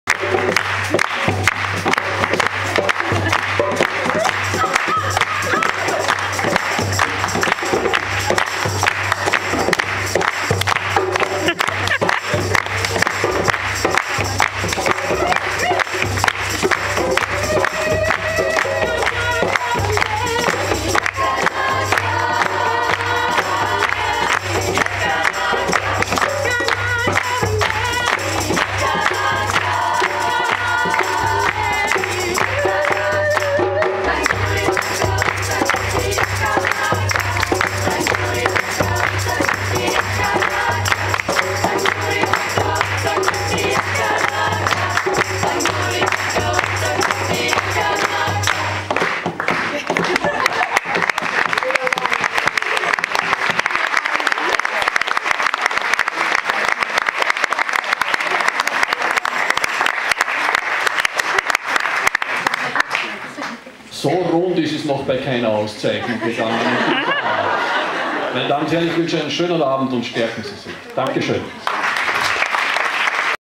Sr. Marco Gneis - Romero-Preis - ORF
Sr. Marco Gneis erhielt den Erzbischof-Oscar-Romero-Preis der Katholischen Männerbewegung, für ihren  40jährigen Einsatz im Selbsthilfezentrum Jabulani in Südafrika. Der Preis wurde ihr am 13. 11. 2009 im ORF-Studio Burgenland in Eisenstadt feierlich überreicht. Ein Fest, wie es nach Aussagen der ORF-Leute bei einer Ehrung noch nie da gewesen ist! Afrika und Österreich haben sich in Sr. Marco harmonisch vereint, zur Ehrung war alles vertreten. Mit ihrer liebevollen, humorvollen und doch resoluten Art Marco begeisterte sie alle Anwesenden restlos. Freunde aus nah und fern, besonders auch aus ihrer Heimatgemeinde Rattersdorf freuten sich mit ihr. Tanz, Musik und viel Herzlichkeit von allen Seiten, besonders von Sr. Marco. Aber auch der Botschafter von Südafrika war zu Marcos größter Freude gekommen. Liebe Marco, allen Segen für Deine weitere Arbeit, weiterhin soviel Schwung und Liebe zu Deinen Leuten. Alle Kraft, die Du brauchst, ist in Dir! Deine Gucki.